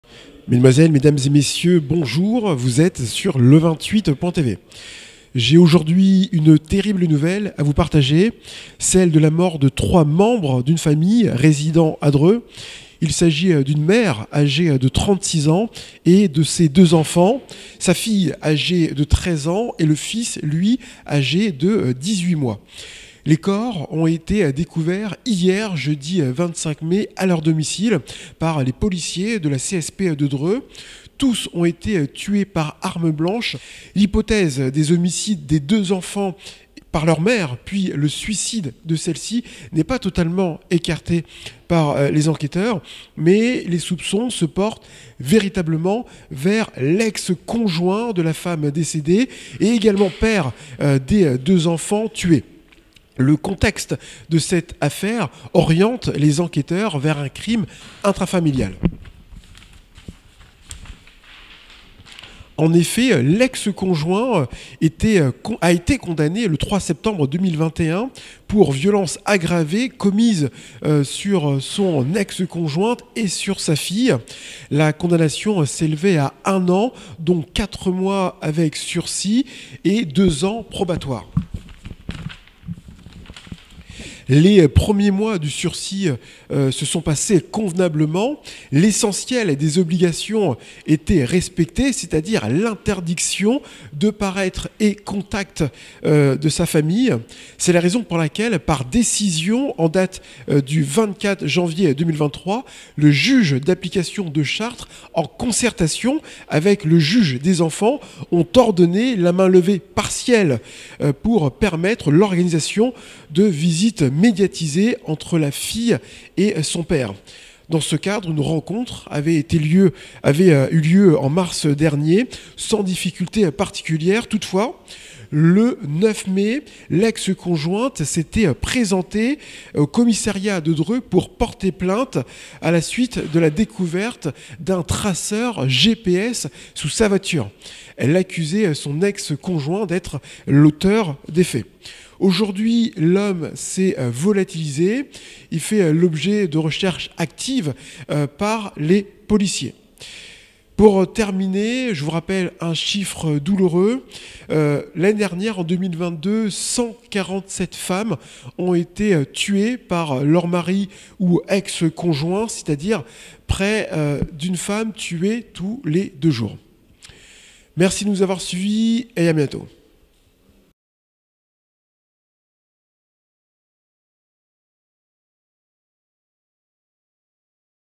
Triple homicide : Une femme et ses deux enfants retrouvés morts à leur domicile
Inquiété par l’absence de réponse de son amie avec laquelle il avait rendez-vous et ne répondant ni au téléphone ni à la porte, un homme a appelé les pompiers jeudi 25 mai à 09h. Ces derniers ont forcé l’entrée et ont fait la macabre découverte des corps sans vie d’une mère et de ses deux enfants âgés de 13 ans et 18 mois. Dépêchés sur place les policiers de la CSP de Dreux ont alors contacté le parquet de Chartres compte tenu de l’aspect criminel de la scène. Les trois victimes présentaient une blessure par arme blanche. Les soupçons des enquêteurs se portent principalement vers l’ex-conjoint qui avait été condamné le 3 septembre 2021 par la justice pour violences aggravées sur sa conjointe et sa fille, à 1 an de prison. Après huit mois de détentions il était ressorti libre dans le cadre du sursis probatoire. L’ex-conjoint est aujourd’hui activement recherché par la police.
Une cellule psychologique à été ouverte au collège privé Saint-Pierre Saint-Paul à Dreux ou était scolarisé la jeune fille assassinée.